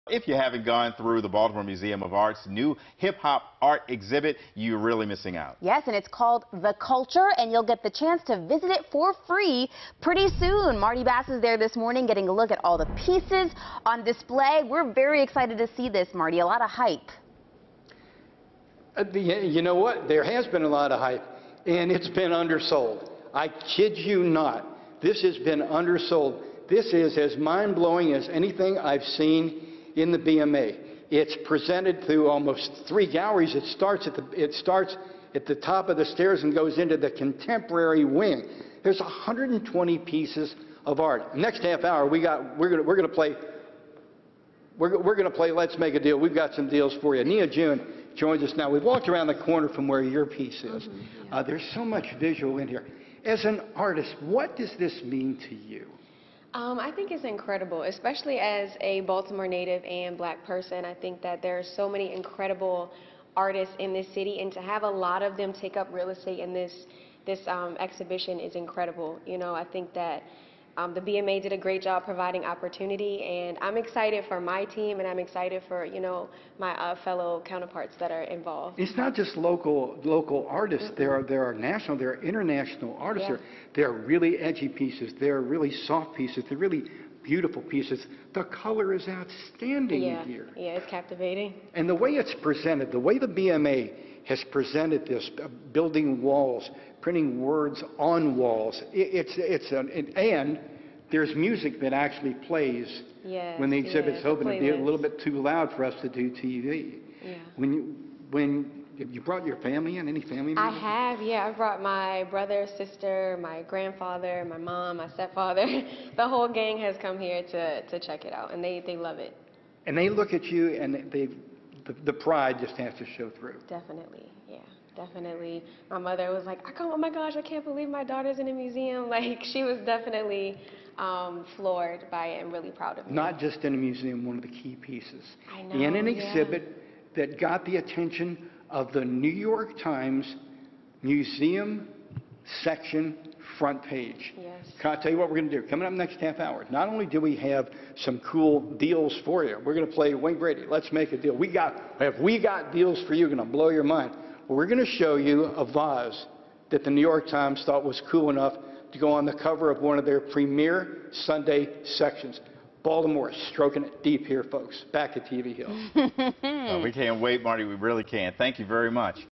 Where's Marty? With artist Nia June at the BMA's newest exhibit
Baltimore-based artist Nia June's work The Unveiling of God / a love letter to my forefathers is in the show and being screened on May 25